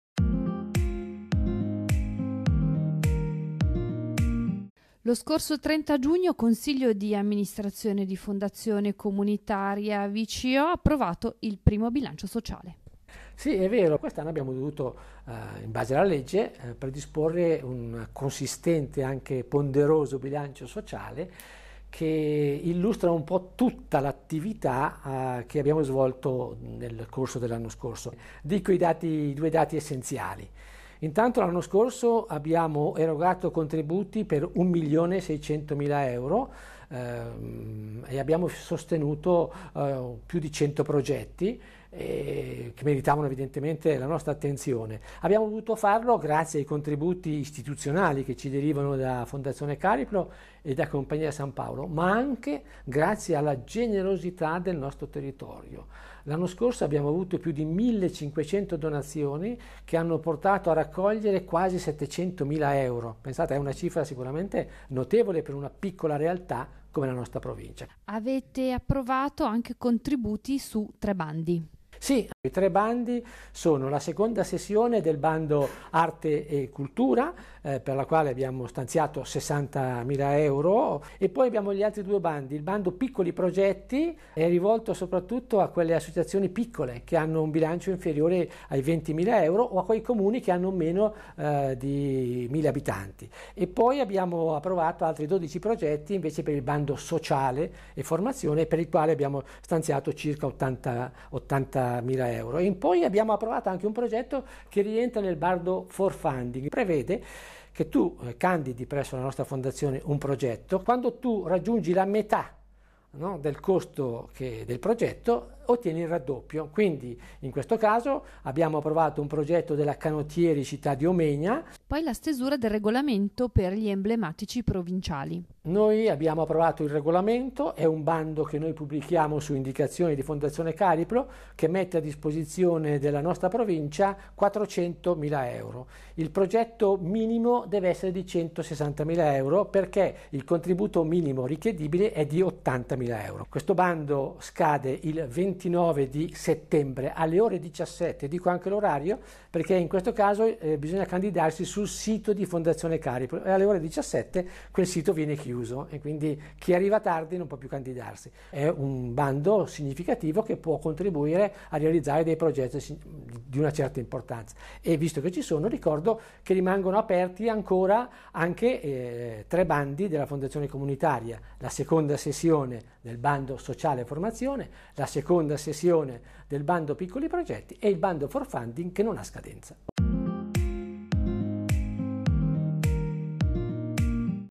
Donare per Crescere, Fondazione VCO: Bilancio Sociale 2022, contributi ed Emblematici Provinciali
Venerdì 30 giugno, presso la sede del Coordinamento Provinciale della Protezione Civile del VCO, si è riunito il Consiglio di Amministrazione della Fondazione Comunitaria del VCO Ente Filantropico che ha approvato il Bilancio Sociale 2022, l’erogazione di contributi per un totale di 199.235 euro e il Regolamento del Bando Emblematici Provinciali 2023.

BILANCIO SOCIALE 2022
Il documento fotografa nel dettaglio l’attività svolta dalla Fondazione Comunitaria nel corso del 2022. I dati più significativi riguardano l’attività erogativa svolta dall’ente che lo scorso anno ha deliberato contributi per un totale di 1 milione e 653 mila euro, a favore di 165 progetti, attraverso i bandi, le erogazioni da Fondi (sono 68 quelli costituiti presso la Fondazione) e con il sostegno a progetti particolari.
Questa attività erogativa è stata resa possibile dai contributi istituzionali di Fondazione Cariplo e Fondazione Compagnia di San Paolo, ma anche dalle 1.546 donazioni ricevute, frutto di campagne promosse sul territorio e che hanno portato a raccogliere 698.584 euro.

CONTRIBUTI DELIBERATI
Il Consiglio di Amministrazione della Fondazione ha approvato 37 progetti, deliberando contributi per un totale di 199.235 euro.
Per la seconda sessione del Bando Arte e Cultura sono stati approvati 10 progetti per un totale di 60.000 euro (nella prima sessione i progetti approvati erano stati 23, con contributi per 120.000 euro).

Per il Bando Sociale e Formazione sono 12 i progetti che sono stati approvati nella prima sessione, per un totale di 79.610 euro.
Anche per il Bando Piccoli Progetti sono 12 i progetti approvati nella prima sessione per un totale di 35.790 euro.
Infine, sul Bando For Funding sono stati approvati 3 progetti. A fronte di ogni raccolta fondi le donazioni varranno raddoppiate dalla Fondazione. Il totale dei tre raddoppi è pari a 23.835 euro.

Tutti gli esiti dei Bandi sono pubblicati e visibili sul sito della Fondazione

EMBLEMATICI PROVINCIALI
Il Consiglio di Amministrazione ha approvato il regolamento del Bando Emblematici Provinciali con il quale Fondazione Cariplo mette a disposizione della nostra provincia un budget di 400.000 euro. L’importo minimo ammissibile per progetto è fissato in 160.000 euro, con un contributo minimo richiedibile di 80.000 euro.
La scadenza per la presentazione delle domande sul sito di Fondazione Cariplo è fissata per le ore 17 di venerdì 29 settembre 2023.

I progetti pervenuti saranno valutati dal Cda della Fondazione Comunitaria del VCO nella seduta di mercoledì 25 ottobre e quindi proposti per il finanziamento a Fondazione Cariplo.